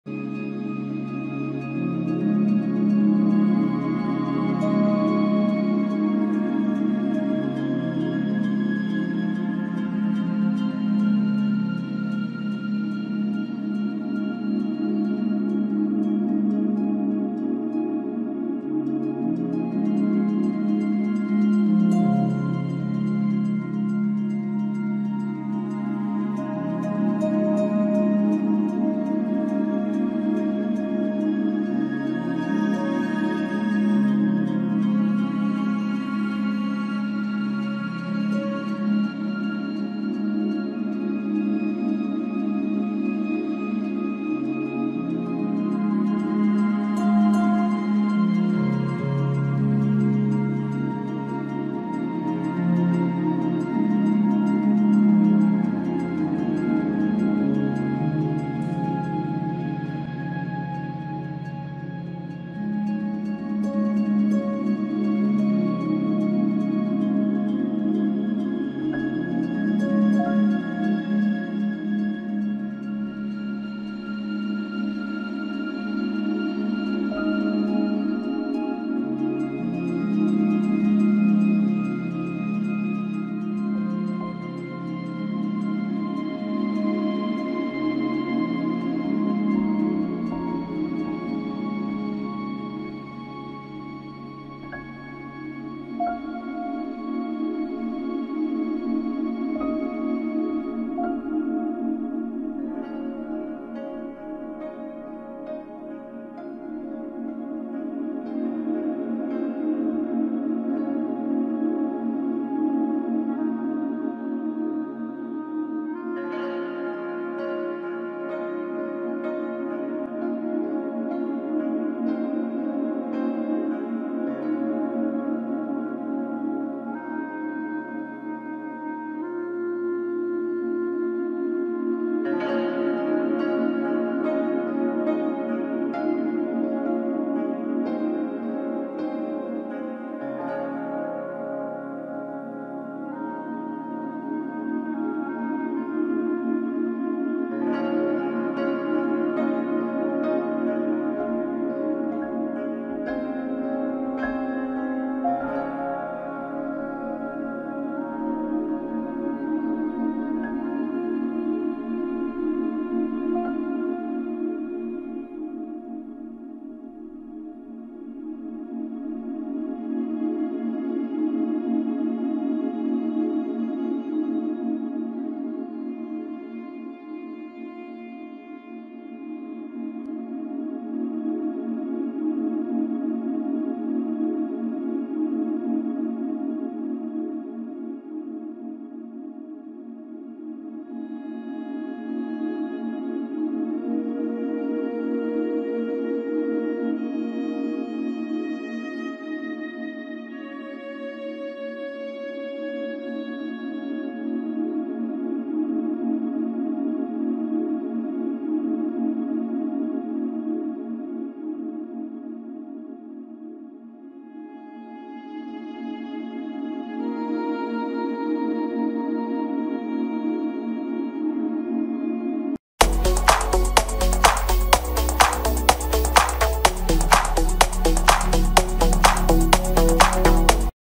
How the lands and coast lines of the earth can be seen from spaceমহাকাশ থেকে পৃথিবীর দেশ গুলো কেমন
These high-res time-lapse sequences captured by astronauts aboard the International Space Station give us a beautiful and clear view of some well-known coastlines and countries around the world.
Rating No mature content